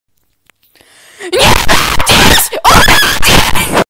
Birthday animation and I changed my name again
the voice was lent by LovelyCookie sub to herrrrr
I would record my voice but it sucks
thank you to all who remembered ❤️
ily potter und kiki
Also she says "Nie mam dziś urodzin" which means "I don't have a birthday today" xd k bye

Ps. I changed my name bc I didn't like the previous one
You can call me Gallow right now or something
But you can call me whatever you want